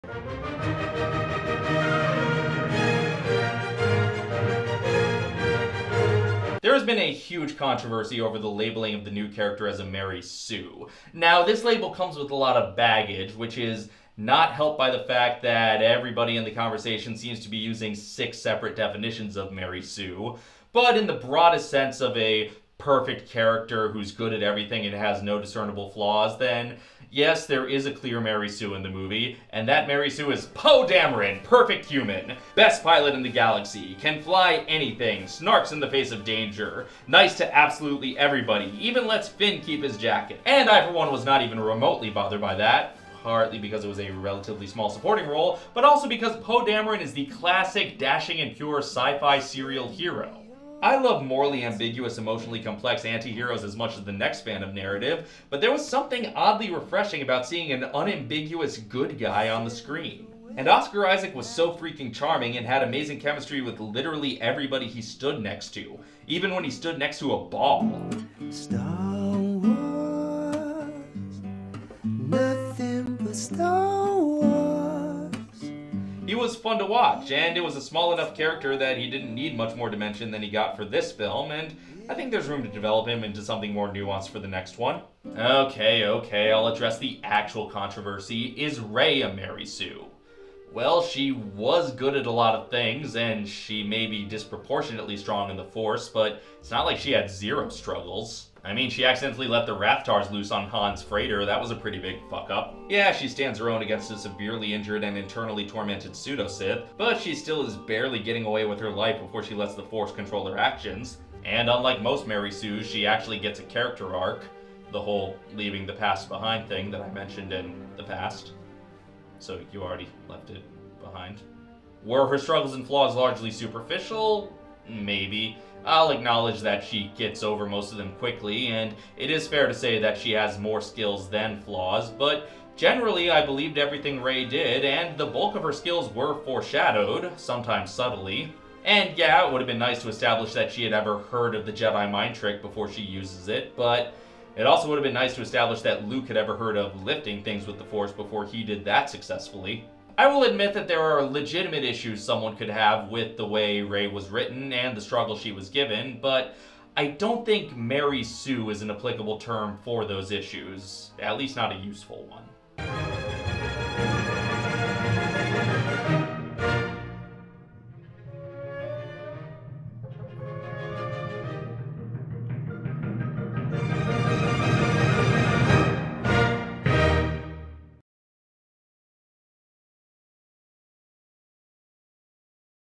Does THE FORCE AWAKENS Have A Mary Sue? The Best of The D-List
This kinda stopped applying when the other two sequels came out, but still!

From THE D-LIST: 11 OBSERVATIONS ABOUT THE FORCE AWAKENS